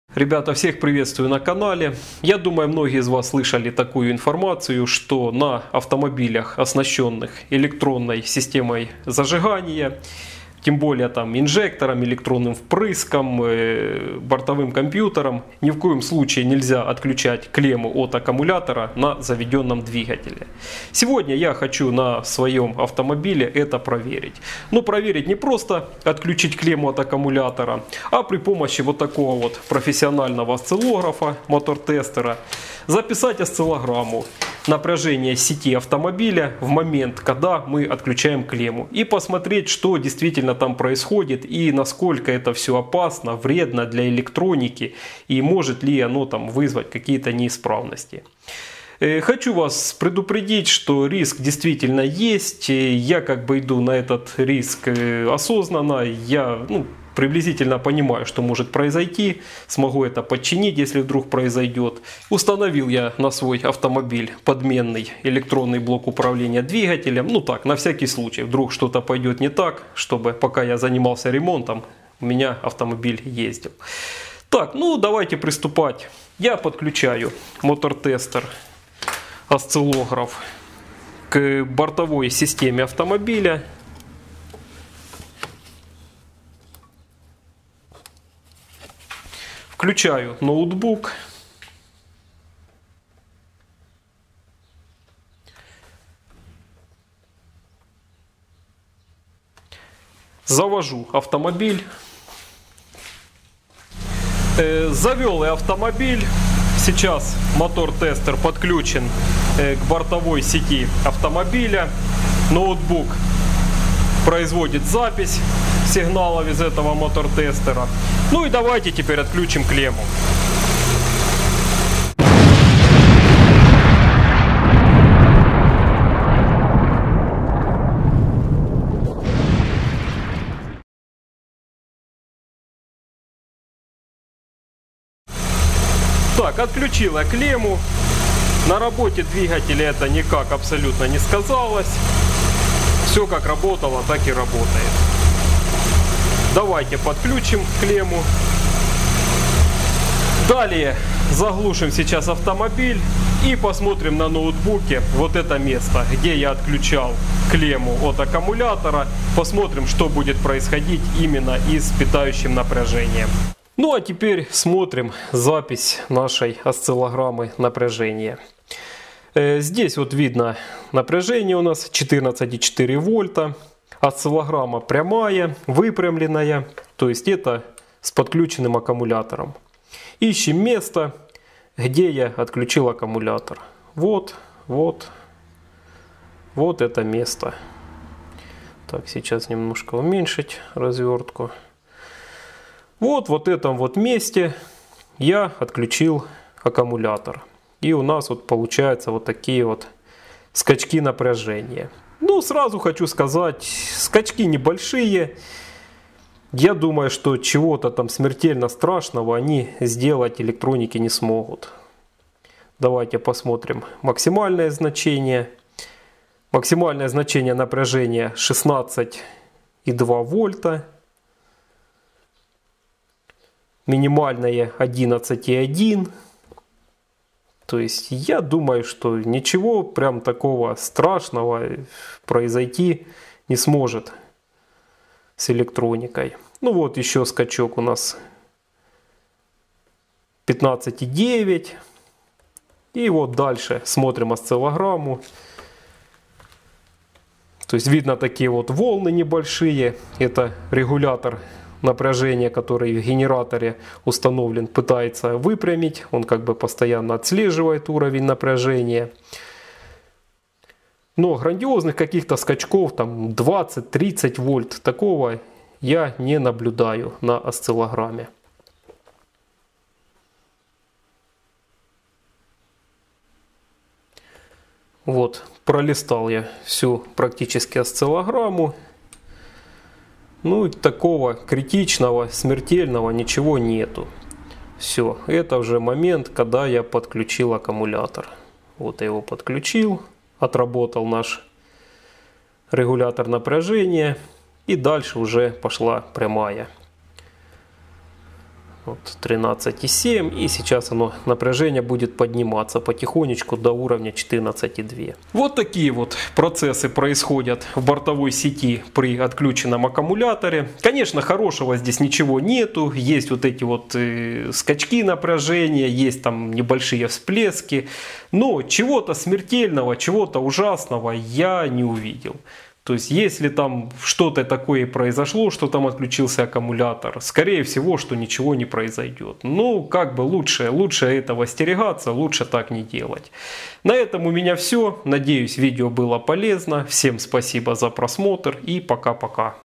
Отключил клемму аккумулятора на работающем двигателе.
Все автолюбители знают, что на современных автомобилях категорически запрещено снимать клемму АКБ на работающем двигателе. Я решил провести эксперимент на своем автомобиле. На заведенном двигателе снять клемму с аккумулятора. На всякий случай, установил запасной электронный блок управления двигателем (ЭБУ). Чтобы понимать, что происходит в сети электропитания автомобиля в момент отключения аккумулятора, я подключил к ней мотор тестер (осциллограф) в режиме записи. На моем авто ВАЗ 2170 (Приора), дополнительно установлено ГБО 4-го поколения, магнитола pioneer, и сигнализация starline, подушка безопасности. В момент эксперимента не один из блоков бортовой электроники не пострадал. Никому не рекомендую повторять данный эксперимент, так как это не безопасно для бортовой электроники автомобиля!